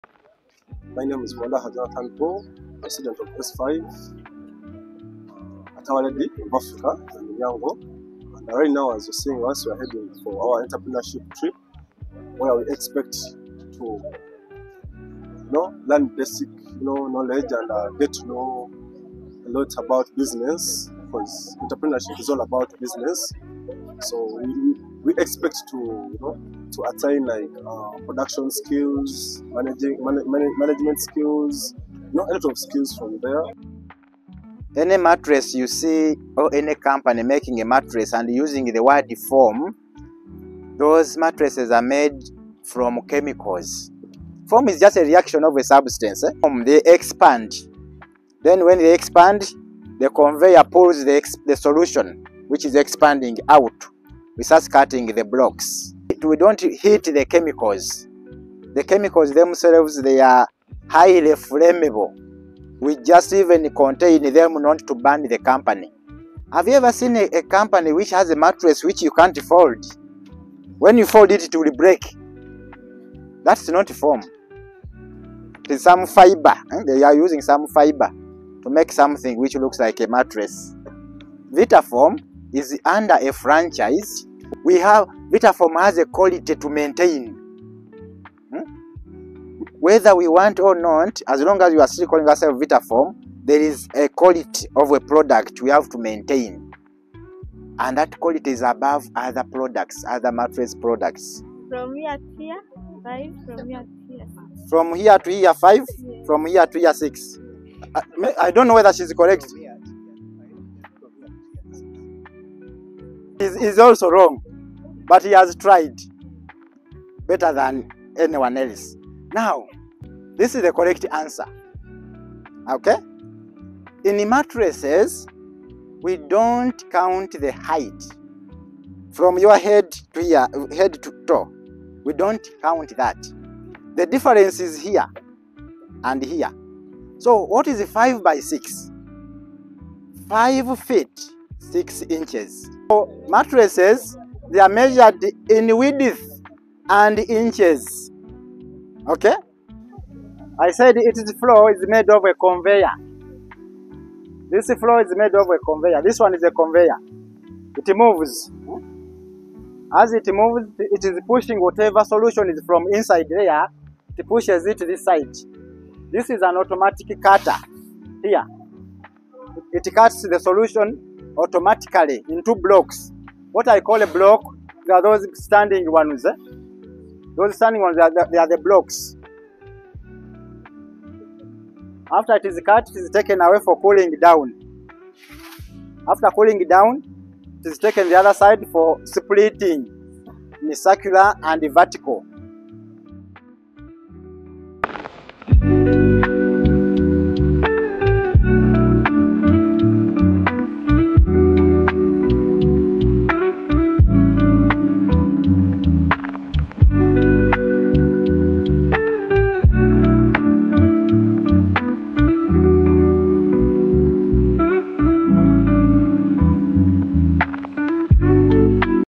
Exploring the art and science of mattress making—an unforgettable business lesson.
Bringing ideas to life! Our Lady of Africa S.S Namilyango’s entrepreneurship students visit Vitafoam Mattresses Company to get an inside look at the mattress-making process. From understanding terms like ‘5 by 6’ to seeing the machinery behind the scenes, this experience was all about discovering the world of production, business insights, and the art of turning raw materials into everyday essentials. Join us on this exciting entrepreneurial journey.